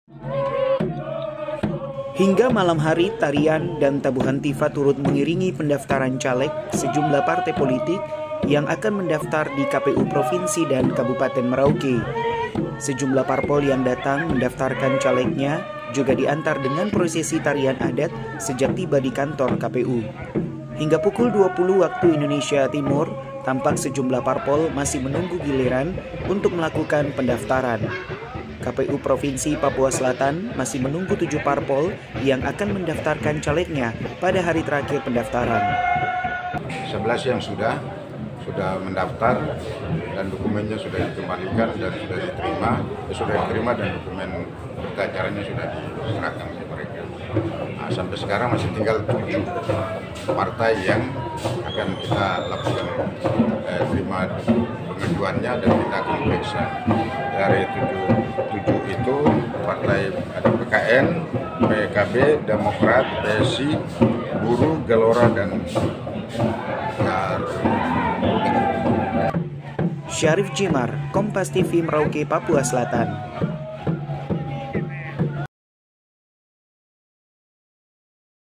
Tunggu Giliran Pendaftaran Caleg Dengan Tarian Adat Marind
MERAUKE, KOMPAS.TV - Hingga malam hari tarian dan tabuhan Tifa turut mengiringi pendaftaran Caleg sejumlah partai politik yang akan mendaftar di KPU Provinsi Papua dan Kabupaten Merauke.

Sejumlah parpol yang datang mendaftarkan calegnya juga diantar dengan prosesi tarian adat sejak tiba di kantor KPU.

Hingga pukul 20.00 WIT tampak sejumlah parpol masih menunggu giliran untuk melakukan pendaftaran.

KPU Provinsi Papua Selatan masih menunggu 7 parpol yang akan mendaftarkan calegnya pada hari terakhir pendaftaran.

Video Editor : Muh.Taksaka

Ruko Soska, Jln. Raya Mandala (samping Taman Mandala) Mandala, Merauke, Papua